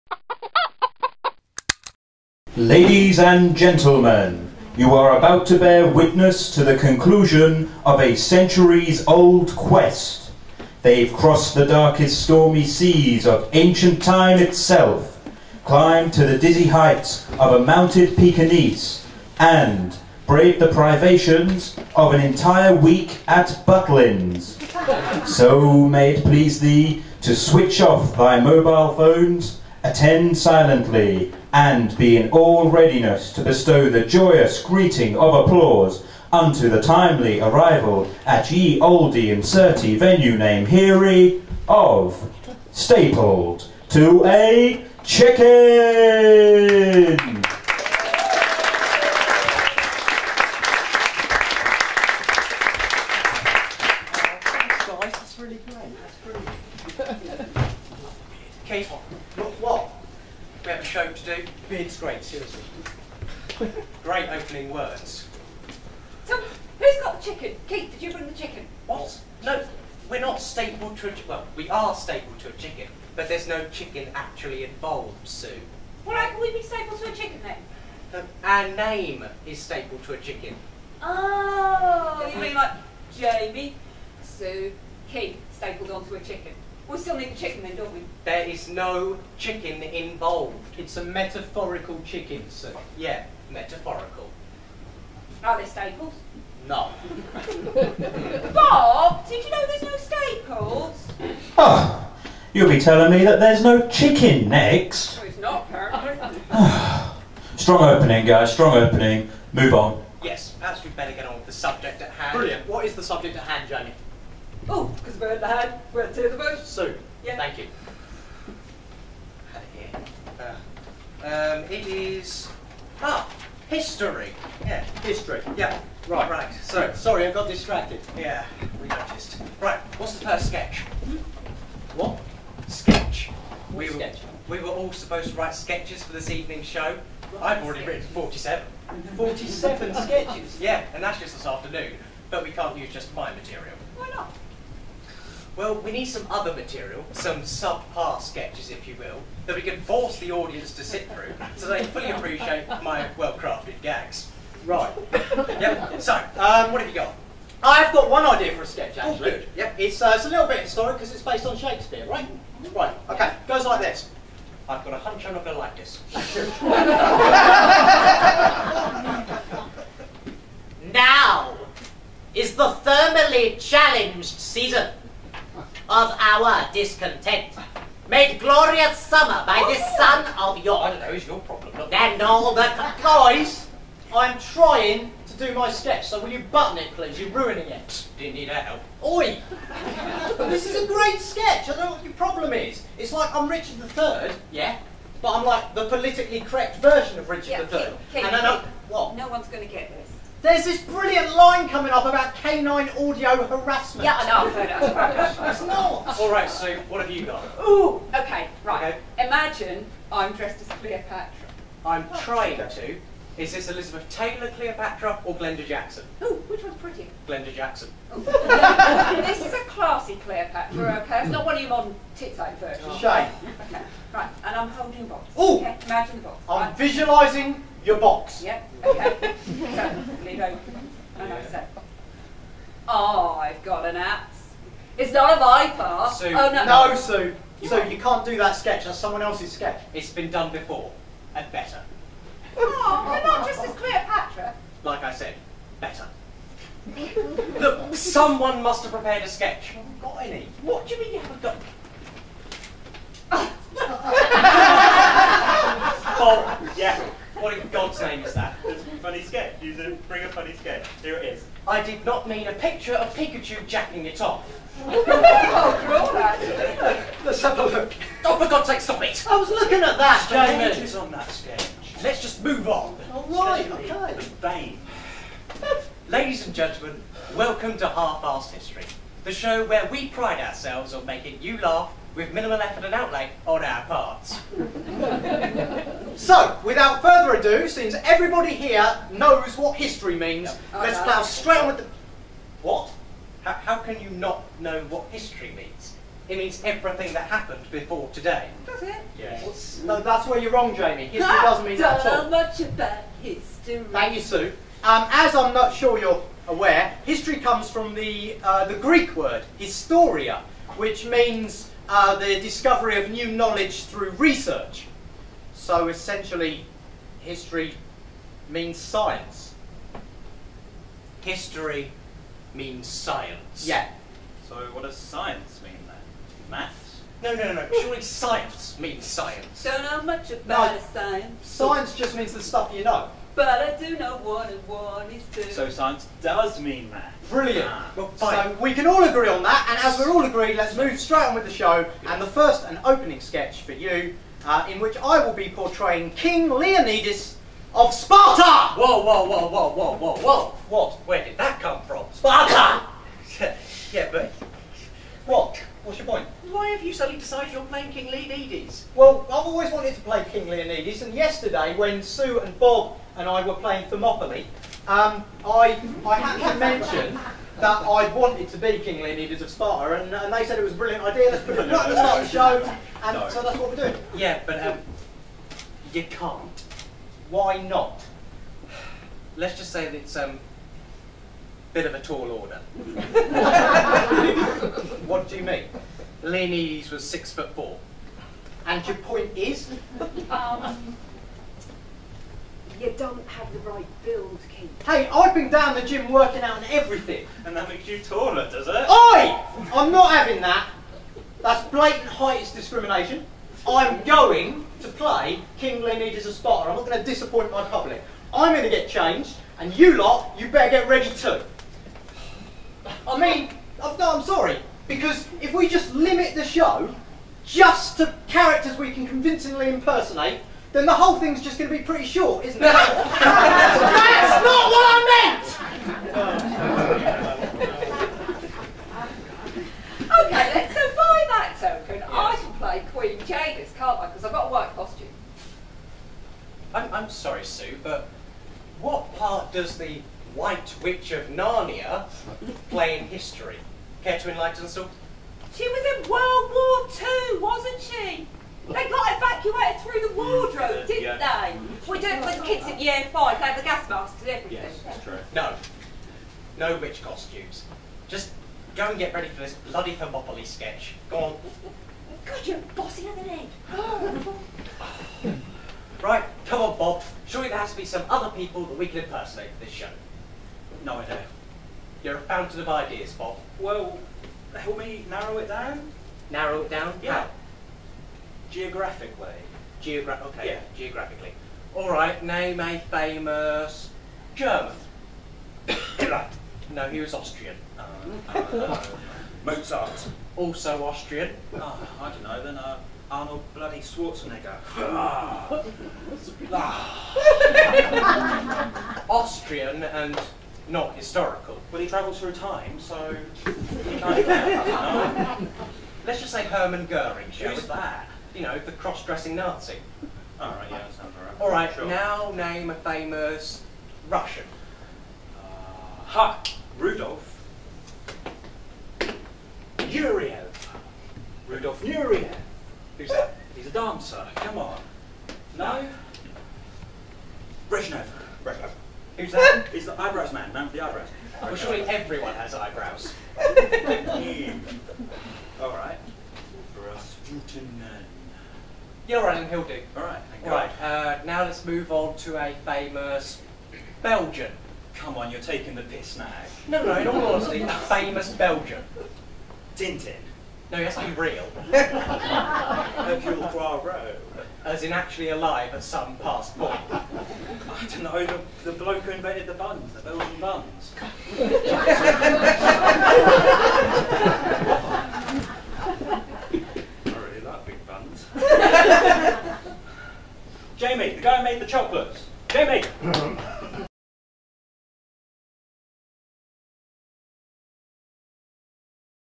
Half-Arsed History Opening
A tag team stand up comedy introduction taken from Stapled to a Chicken's 2014 show "Half-Arsed History". Jamie's written lots of sketches for the show, but it seems nobody else has prepared any funny sketches...